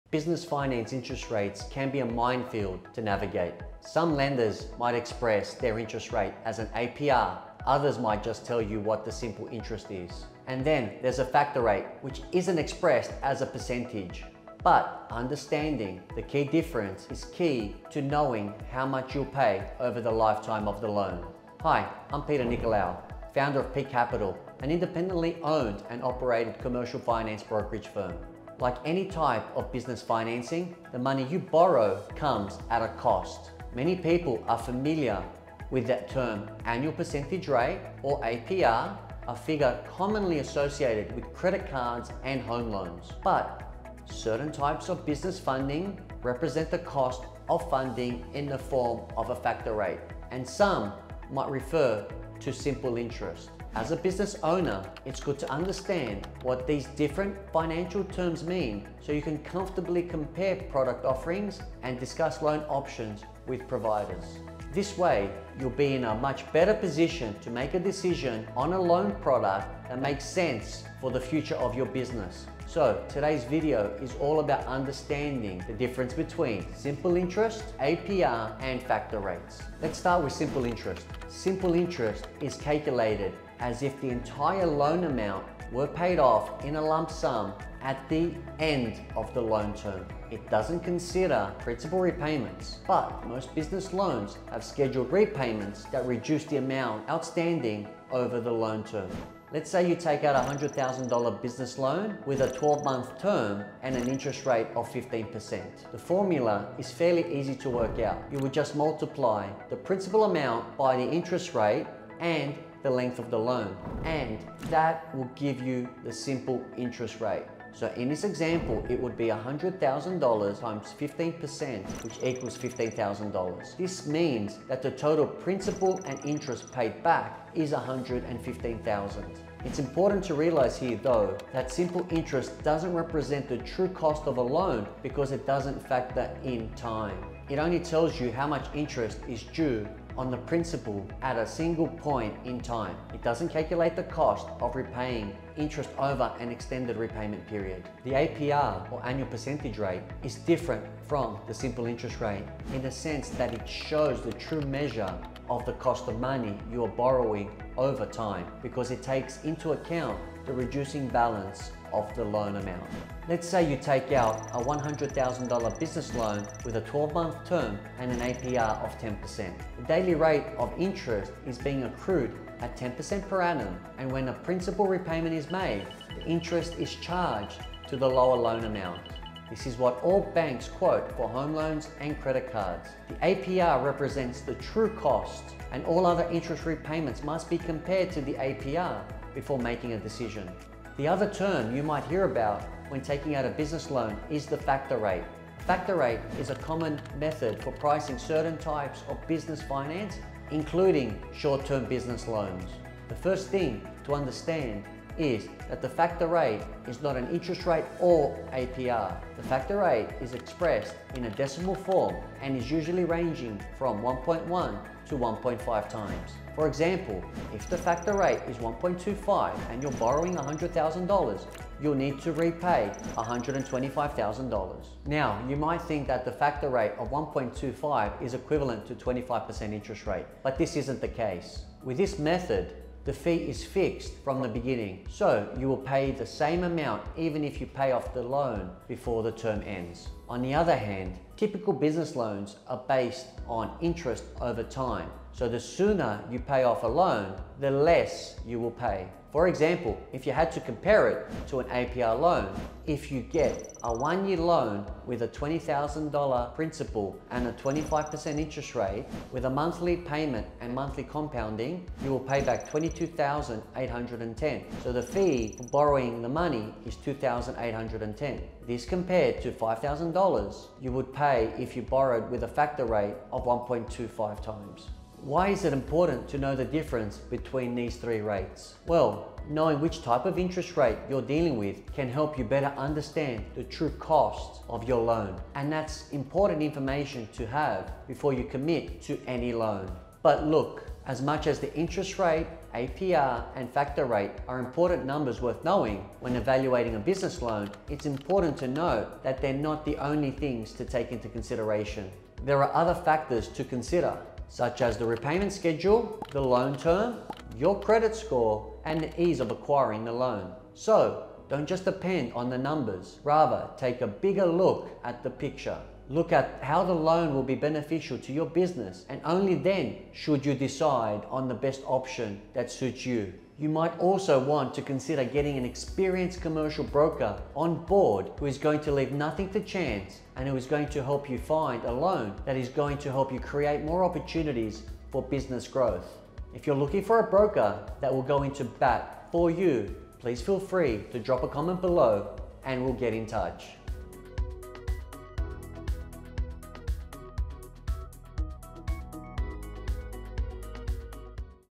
What is the Difference Between Simple Interest, Factor Rate, and APR?
Why should you know the difference between simple interest, factor rate and APR?

Not knowing the difference could have a major impact on how much you pay over the life of the loan.